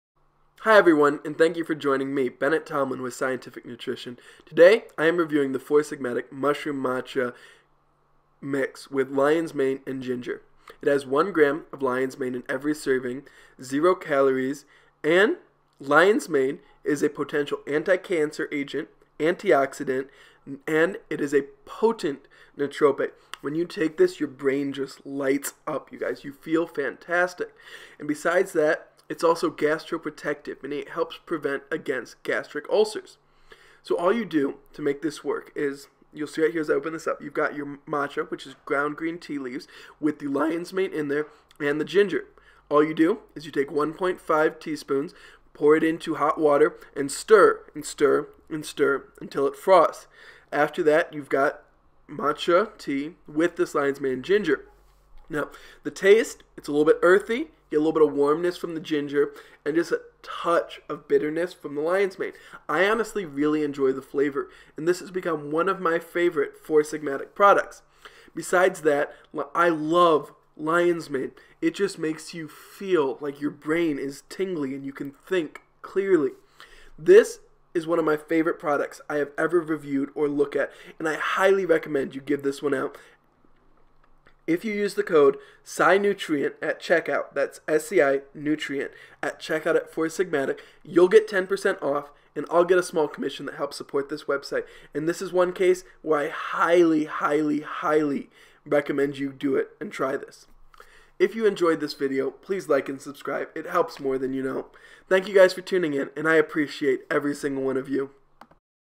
Mushroom Matcha with Lions Mane Review (Four Sigmatic)
Mushroom Matcha Drink Mix with Lions Mane Review (Four Sigmatic)

Hi everyone and thank you for joining me
Bennett Tomlin with scientific nutrition
today I am reviewing the four sig Matic
mushroom much mix with lion's mane and
ginger

It has one gram of lion's mane in
every serving zero calories and lion's
mane is a potential anti-cancer agent
antioxidant and it is a potent nootropic

When you take this your brain just
lights up you guys you feel fantastic
and besides that it's also gastro
protective and it helps prevent against
gastric ulcers so all you do to make
this work is you'll see right here is
open this up you've got your matcha
which is ground green tea leaves with
the lion's mane in there and the ginger
all you do is you take 1.5 teaspoons
pour it into hot water and stir and stir
and stir until it Frost's after that
you've got matcha tea

With this lion's
mane ginger know the taste it's a little
bit earthy get a little bit of warmness
from the ginger and just a touch of
bitterness from the lion's mane I
honestly really enjoy the flavor and
this has become one of my favorite for
sig Matic products besides that well I
love lion's mane it just makes you feel
like your brain is tingly and you can
think clearly.

This is one of my favorite
products I have ever reviewed or look at
and I highly recommend you give this one
out if you use the code Sai nutrient at
checkout that's sei nutrient at checkout
support this website and this is one
case where I highly highly highly
recommend you do it and try this if you
enjoyed this video please like and
thank you guys for tuning in and I
appreciate every single one of you